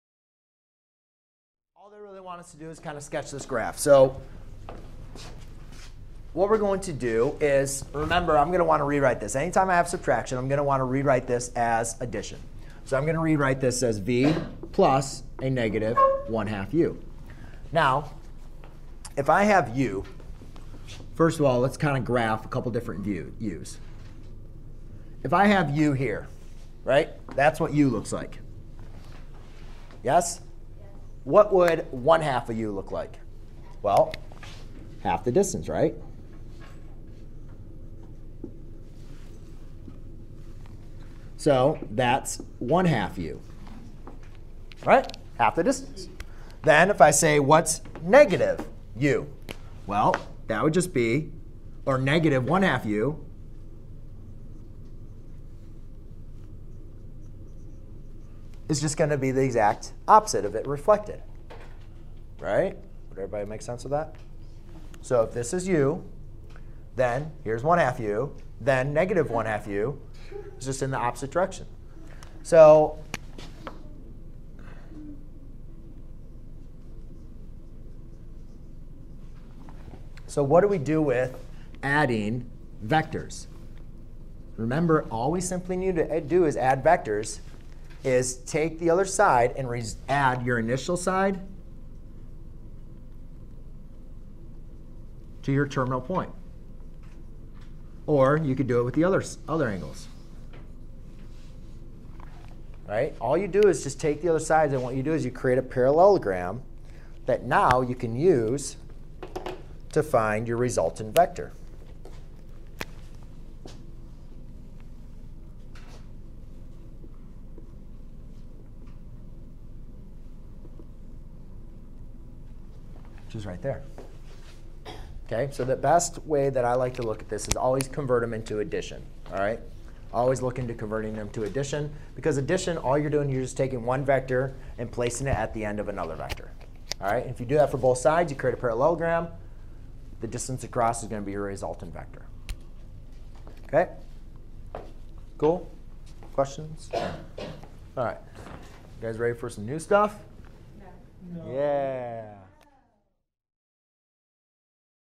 Graph the resultant vector of the difference of two vectors with a scalar
Learn how to determine the resultant vector by adding, subtracting and multiplying vectors by a scalar.  We will also learn how to graph the resultant vectors to show the operations. Vectors can be added, subtracted and multiplied. To add or subtract two or more vectors, we add each of the corresponding components of the vectors. To multiply a scalar to a vector, we simply multiply the scalar to each of the components of the vector.

Note that if you are to add vectors multiplied to scalars, you first multiply the scalar(s) to the vector(s) before adding them.